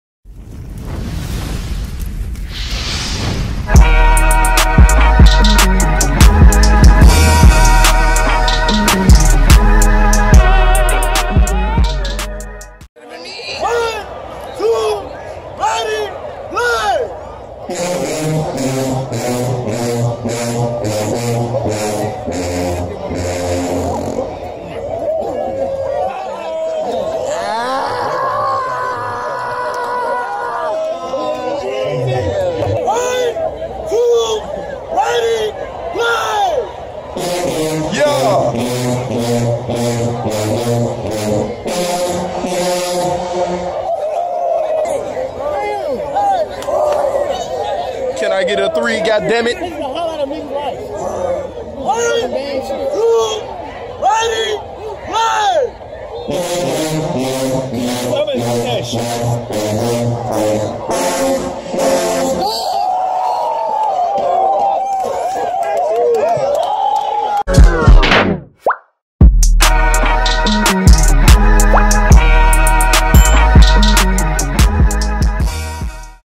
Southern University Human Jukebox Tubas
RAW FOOTAGE OF ONE OF THE BEST TUBA SECTIONS TO REPRESENT THE HUMAN JUKEBOX